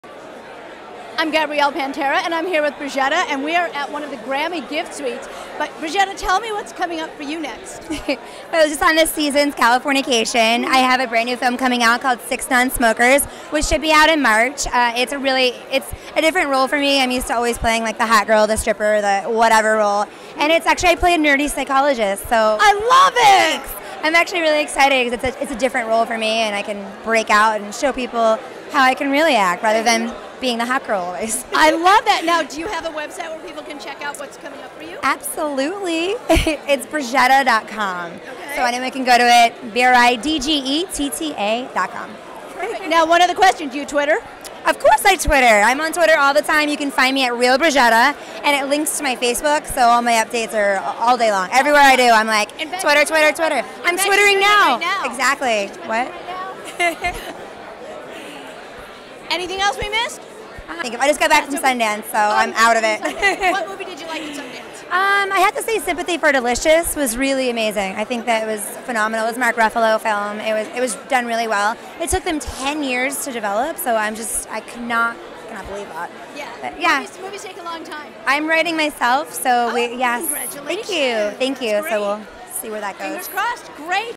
Bridgetta on Gosh!TV
Gabrielle Pantera Interviews Bridgetta in the Serenity gift room before the 2010 Grammys. Talk about her being in Californication and her upcoming film 6 Nonsmokers.